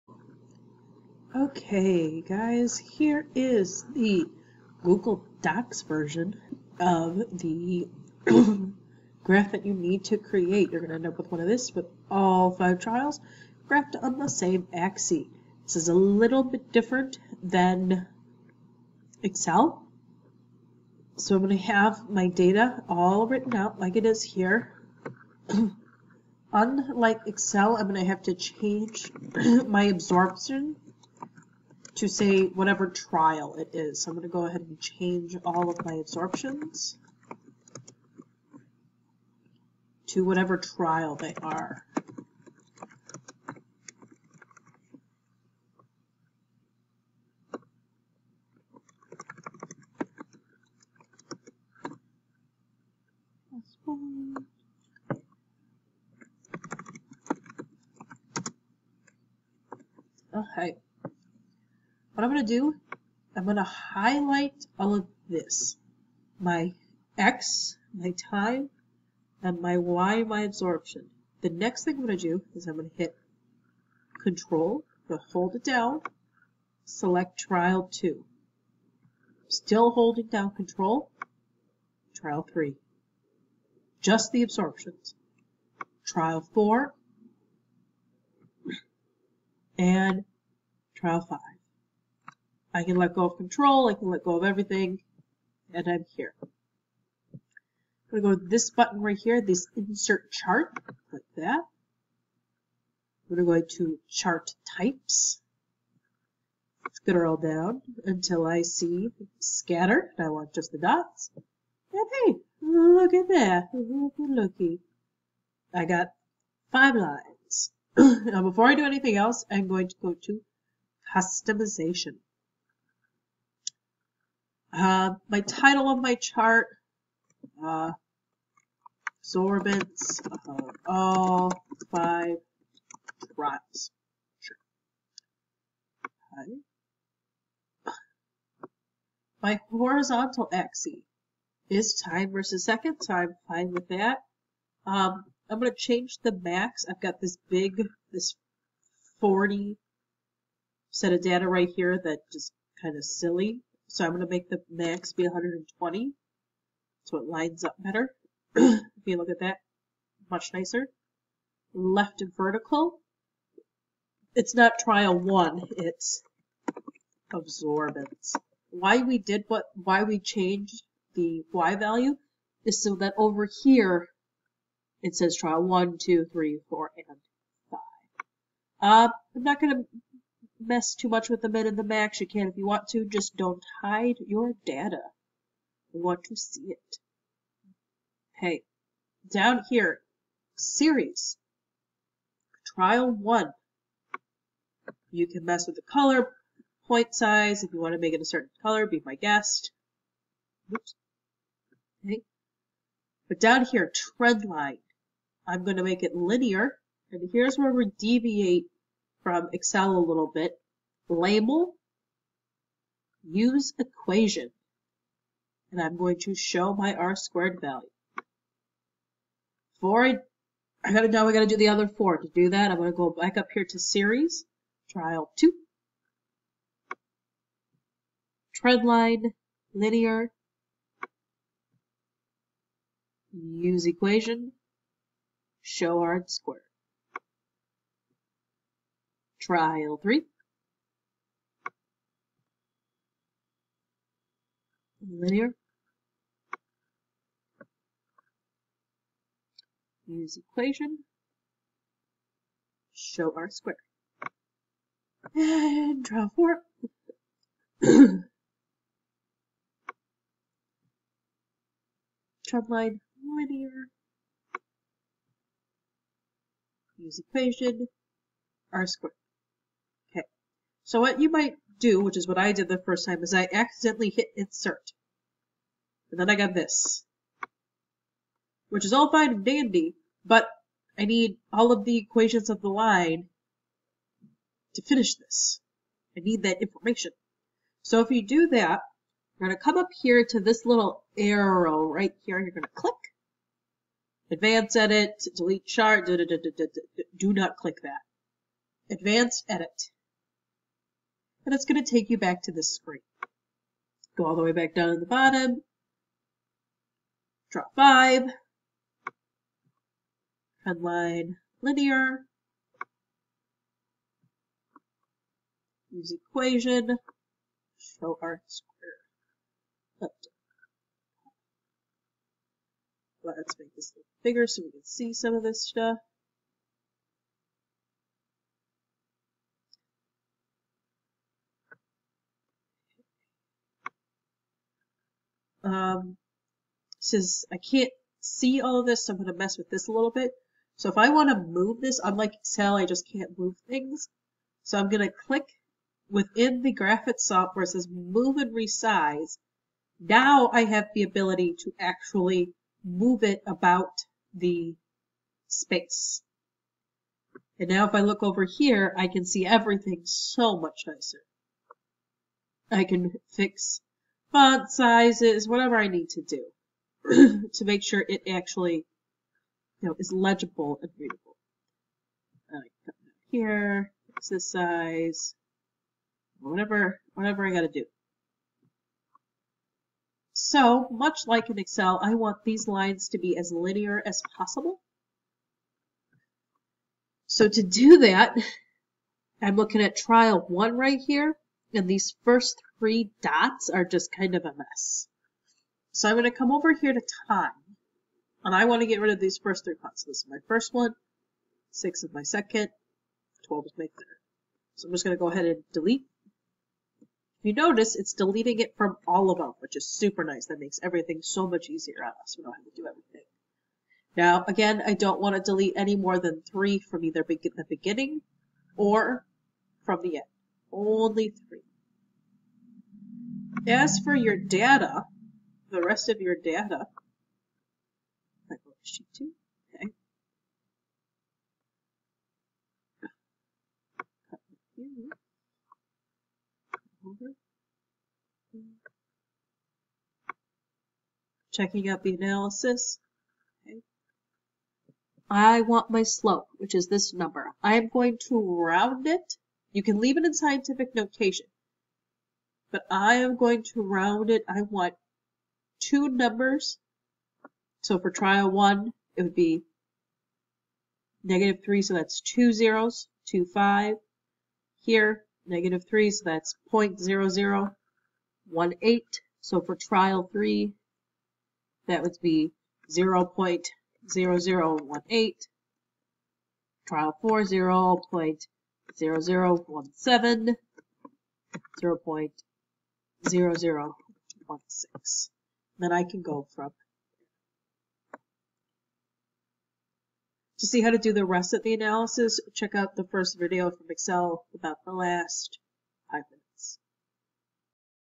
JDHS AP Chemistry Lab 10 Analysis Google Sheets
Here is a video to help you finish your analysis of Lab 10 and your lab report.